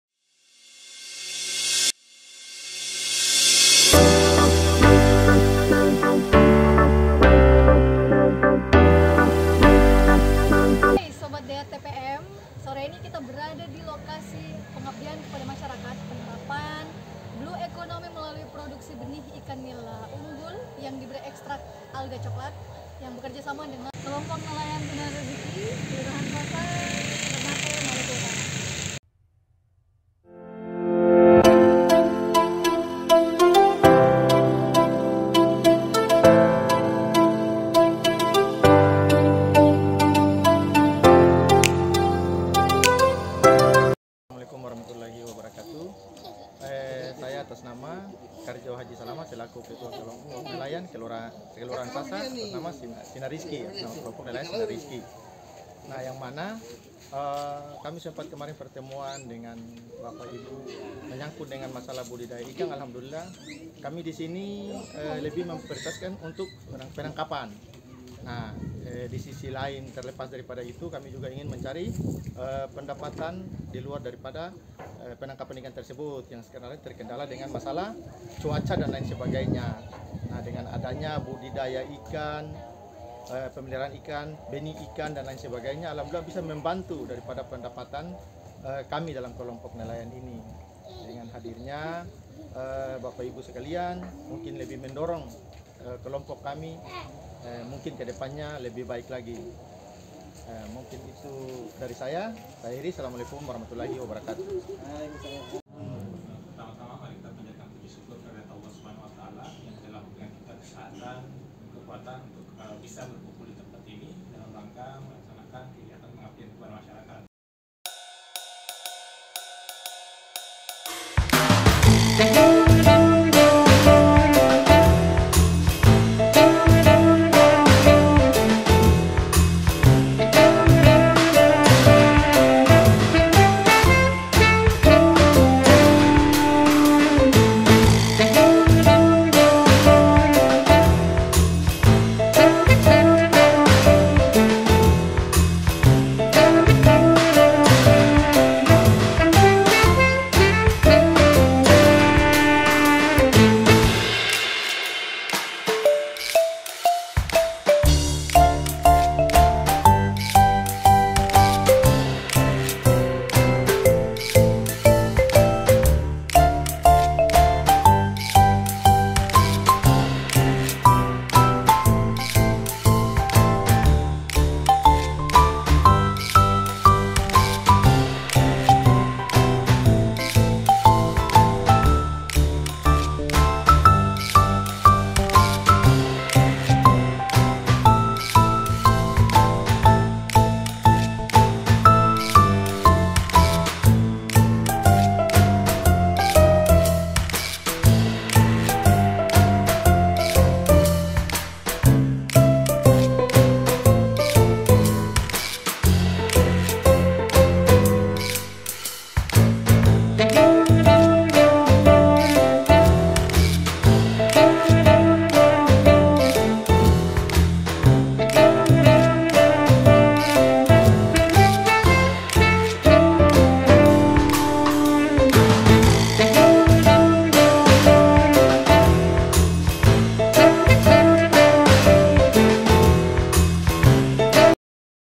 𝐏𝐞𝐧𝐞𝐫𝐚𝐩𝐚𝐧 𝐁𝐥𝐮𝐞 𝐄𝐤𝐨𝐧𝐨𝐦𝐢 𝐌𝐞𝐥𝐚𝐥𝐮𝐢 𝐏𝐫𝐨𝐝𝐮𝐤𝐬𝐢 𝐁𝐞𝐧𝐢𝐡 𝐈𝐤𝐚𝐧 𝐍𝐢𝐥𝐚 𝐔𝐧𝐠𝐠𝐮𝐥 𝐘𝐚𝐧𝐠 𝐃𝐢𝐛𝐞𝐫𝐢 𝐄𝐤𝐬𝐭𝐫𝐚𝐤 𝐀𝐥𝐠𝐚 𝐂𝐨𝐤𝐥𝐚𝐭
Kendala utama yang dihadapi oleh kelompok nelayan Sinar Rezeki adalah hasil tangkapan ikan yang tidak menentu karena faktor cuaca dan kondisi penurunan sumber daya ikan di sekitar pantai Pulau Ternate telah mengakibatkan sebagian besar anggota kelompok mengalami penurunan pendapatan, sehingga dibutuhkan alternatif mata pencaharian tambahan. Kegiatan pembenihan ikan nila telah dilakukan sebelumnya namun belum memberikan hasil yang menguntungkan karena keterbatasan pengetahuan dan keterampilan teknis penanganan induk dan benih ikan nila. Kegiatan PKM ini bertujuan untuk meningkatkan pemahaman dan keterampilan anggota kelompok untuk membudidayakan ikan inila khususnya pembenihan. Ekstrak alga coklat yang kaya kandungan antioksidan, senyawa fenolik dan lainnya telah terbukti sebagai anti bakteri dan parasit, serta mampu mempercepat pertumbuhan bagi ikan nila. Semoga kegiatan PKM ini memberi dampak positif terhadap ekonomi anggota kelompok mitra sasaran,